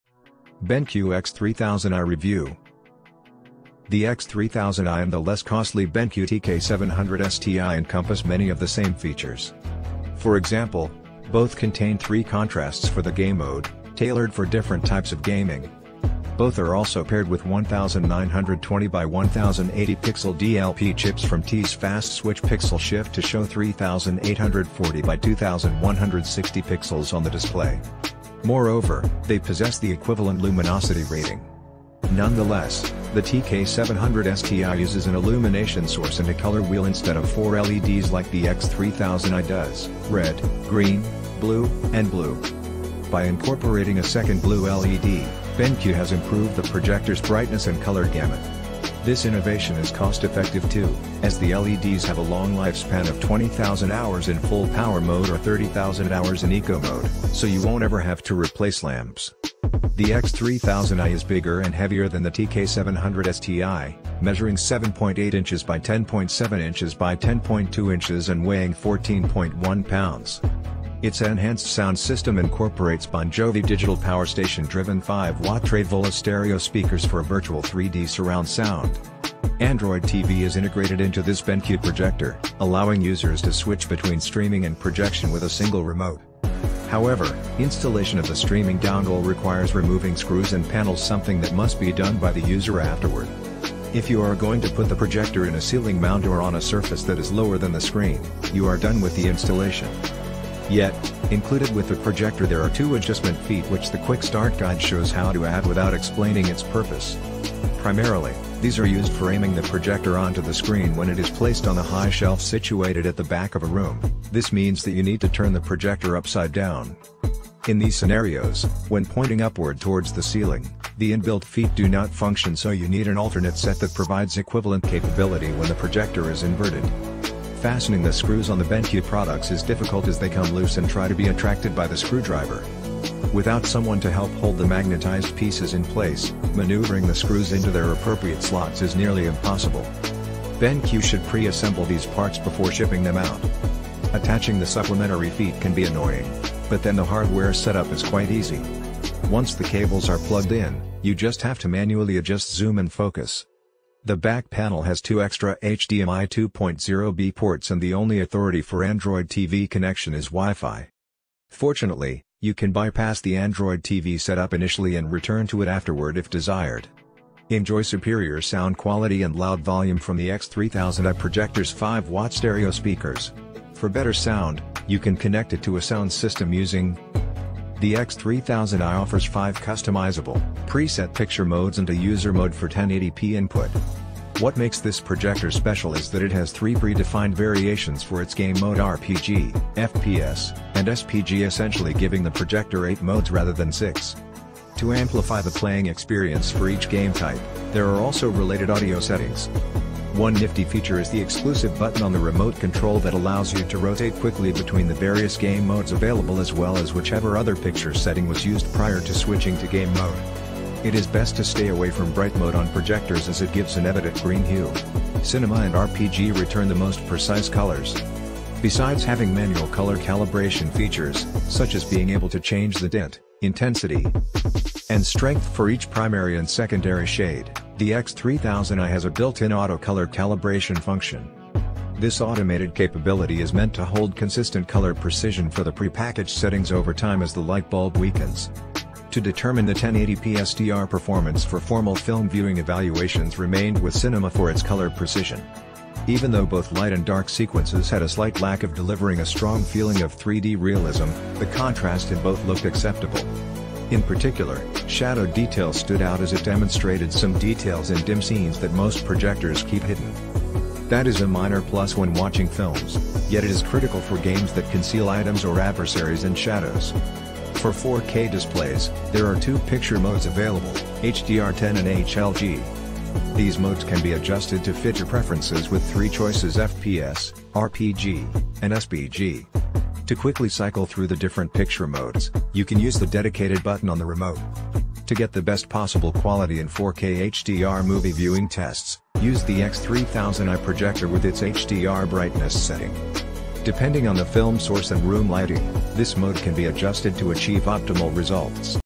BenQ X3000i: Elevate Your Gaming Experience with This Cutting-Edge Monitor
Amazon Product Name : BenQ X3000i

Welcome to our BenQ X3000i review! In this video, we'll be taking an in-depth look at BenQ's latest wireless smart projector. The X3000i offers a range of advanced features, including wireless streaming, voice control, and advanced image processing, all designed to provide a stunning home theater experience. We'll explore its design, performance, and software to give you a comprehensive understanding of what this projector has to offer. From its picture quality to its ease of use and compatibility with popular devices, we'll put the BenQ X3000i through its paces to see how it performs. So, join us as we take a closer look at this powerful projector and find out if it's the right choice for your home theater needs.